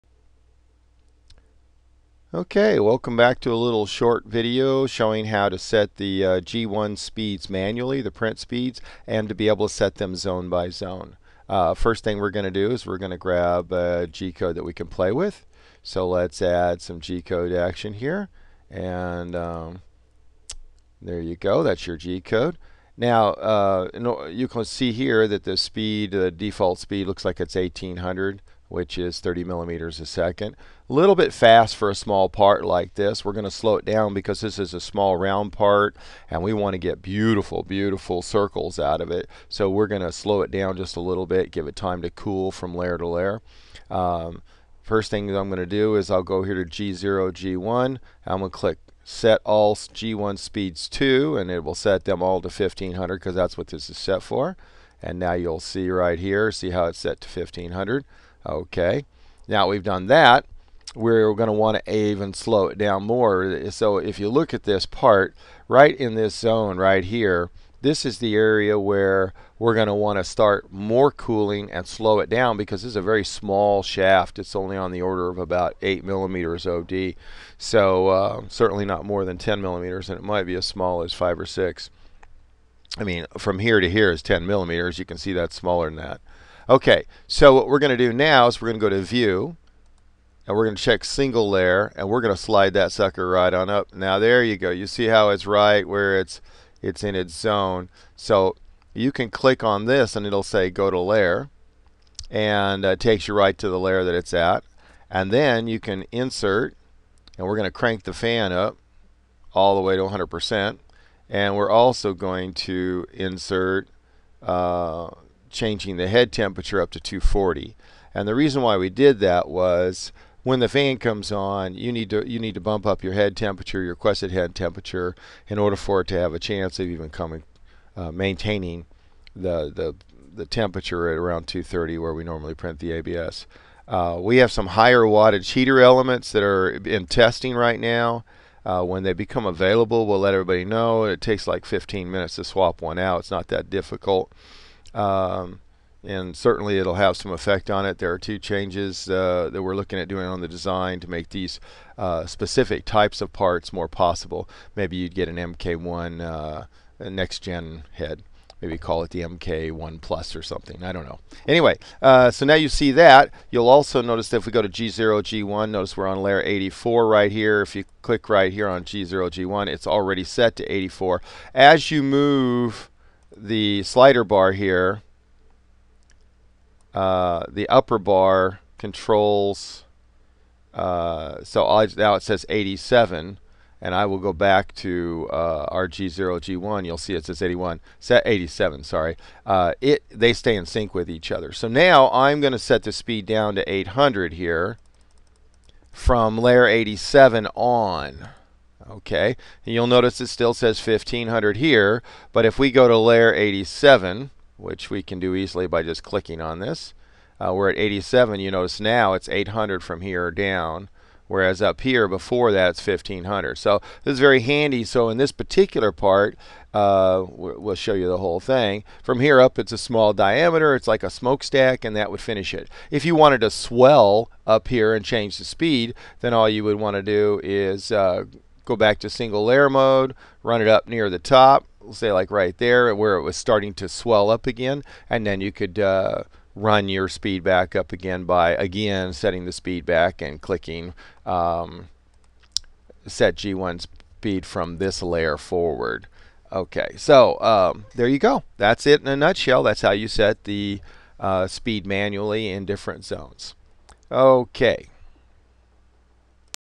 setting print speed and fan manually in repetrel Gcode
Hyrel3d Printer, How to manually change the speed of the print,
whole print, and print Zones.